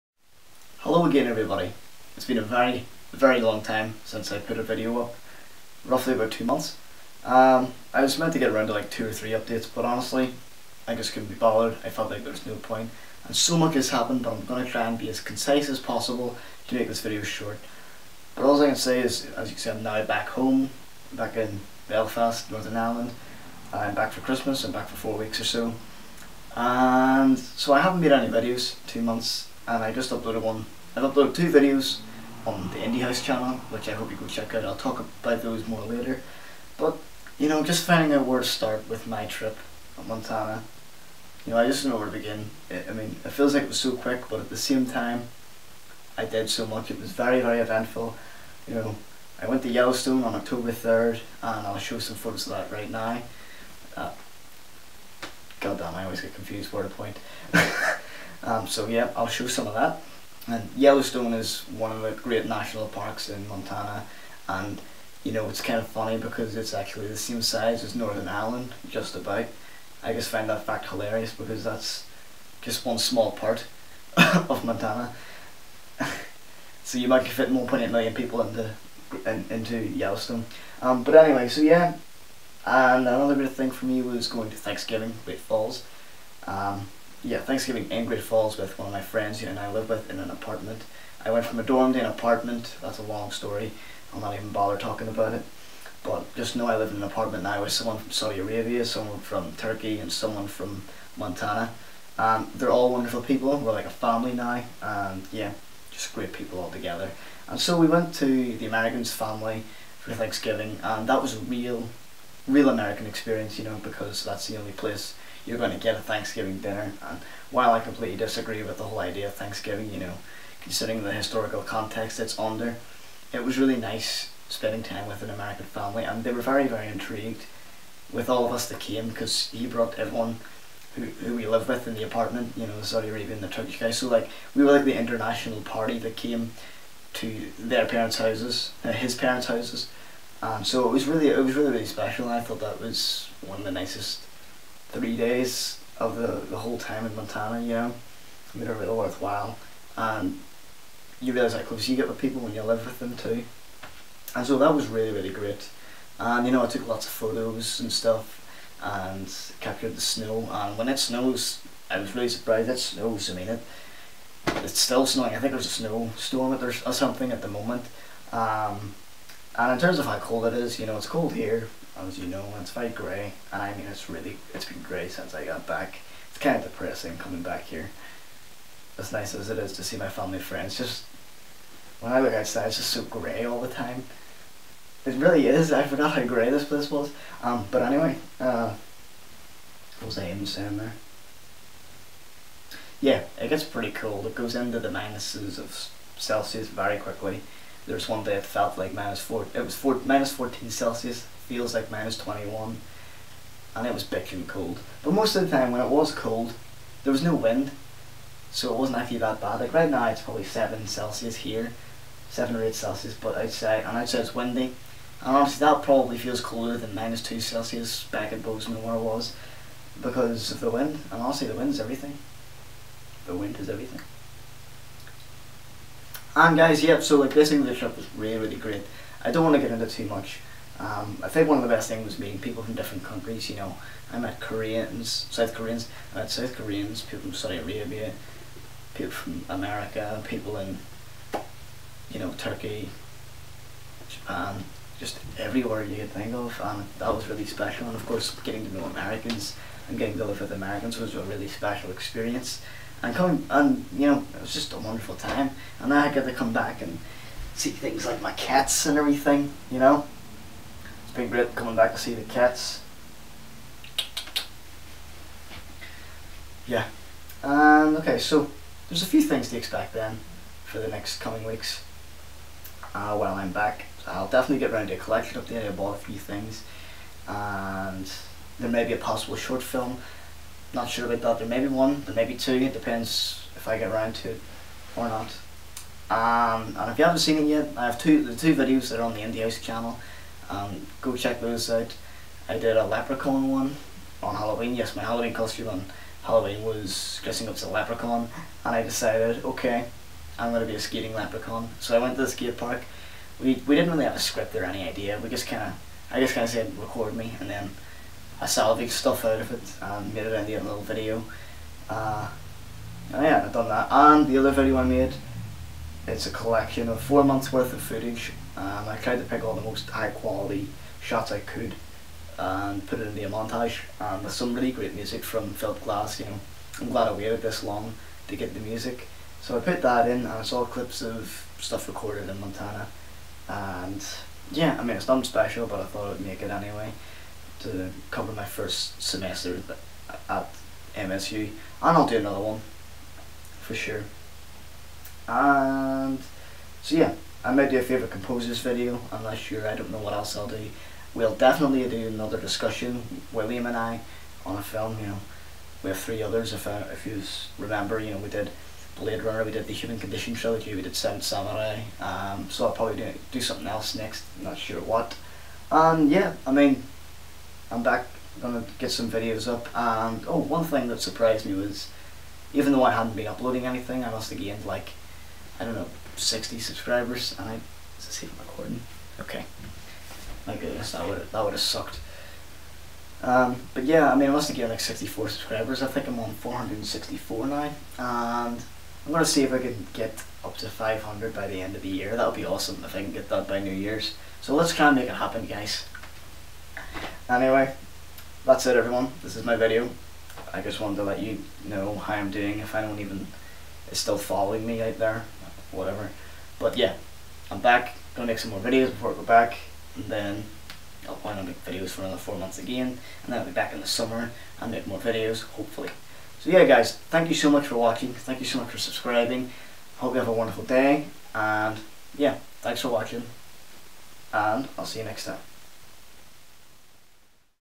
Montana Trip VLOG 3: Channel Update
So I am back in Belfast for Christmas, half way through my study at Montana State University and what a life-changing experience it has been. Excuse the rather bad audio quality on this also. Here's a little update.